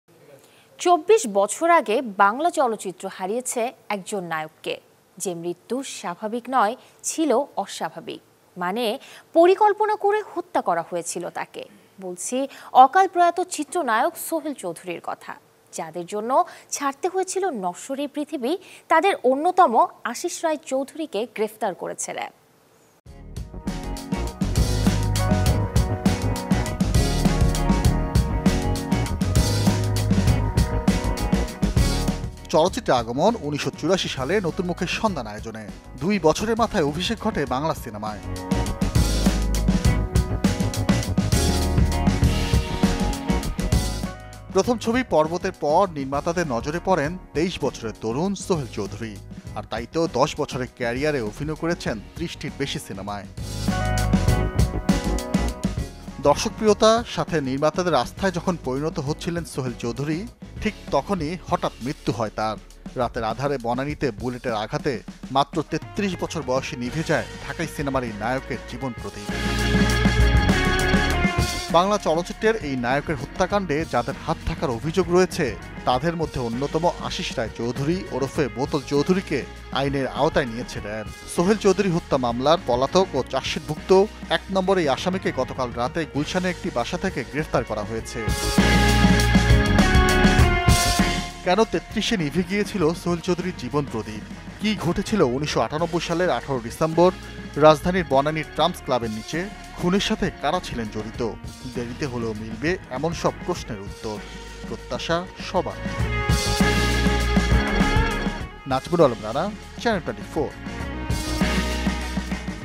কেন ৩৩ বছর বয়সে হ*ত্যা হয়েছিলেন সোহেল চৌধুরী? | Sohel Chowdhury | Channel 24
»» Office Address:
Bangladesh.